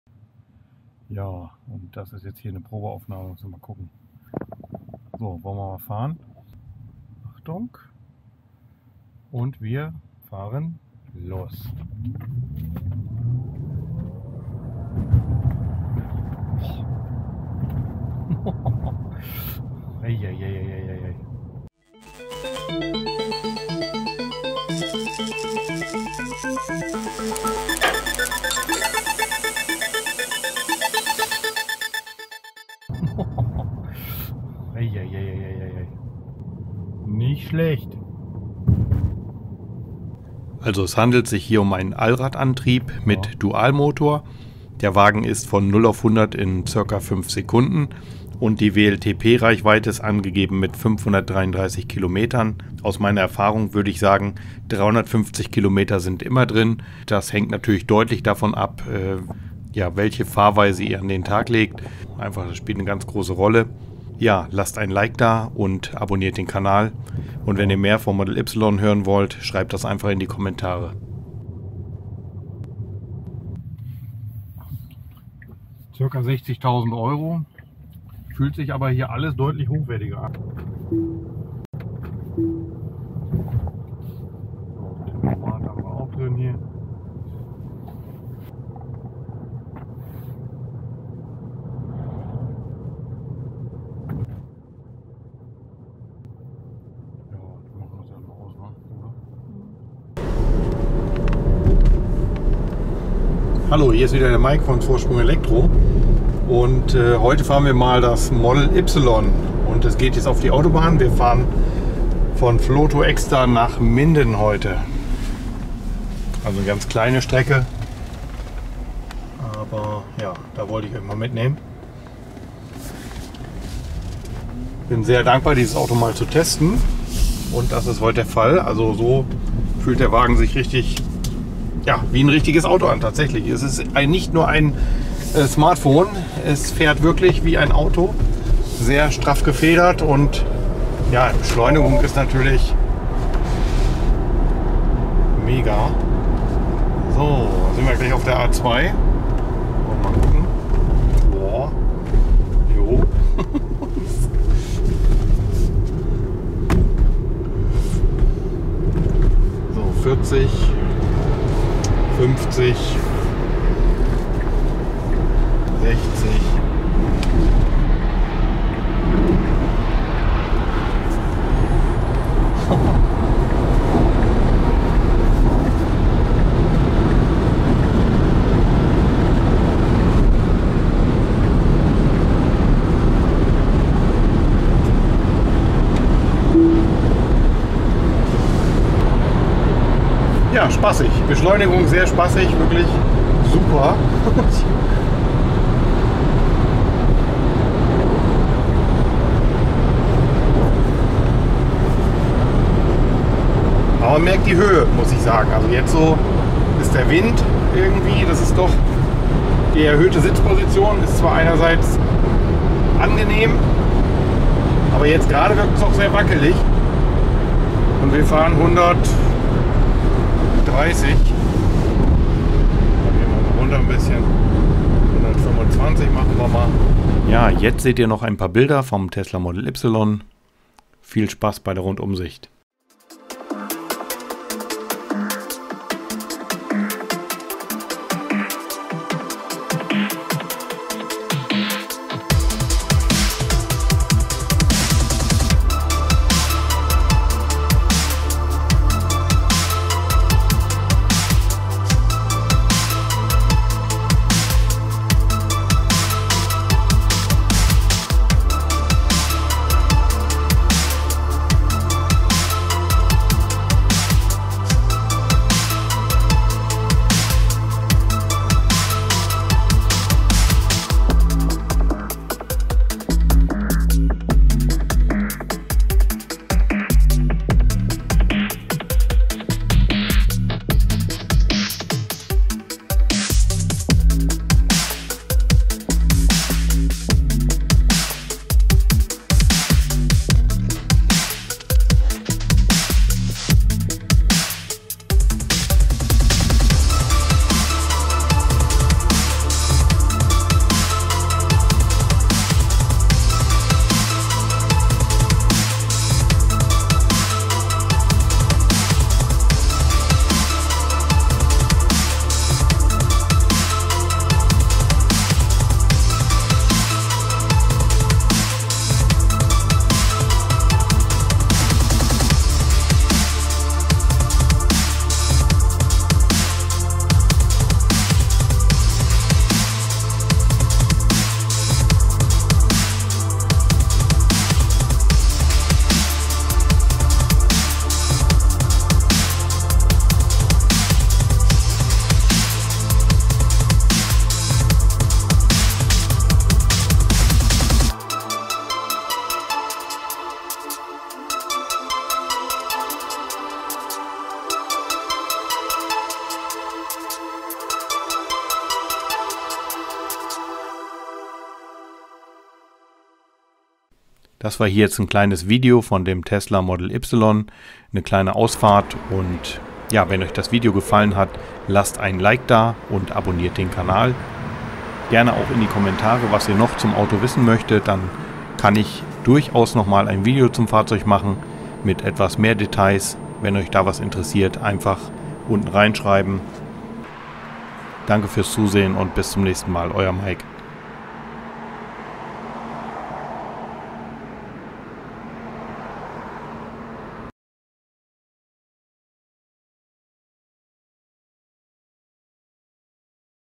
Tesla Model Y erste Ausfahrt und ein Rundgang
Meine erste Ausfahrt mit dem Tesla Model Y.

Beeindruckendes Fahrzeug, sehr gutes Preis-Leistungsverhältnis in diesem Segment! Schreibt gern in die Kommentare was euch noch zum Fahrzeug interessiert...

Also Abo nicht vergessen!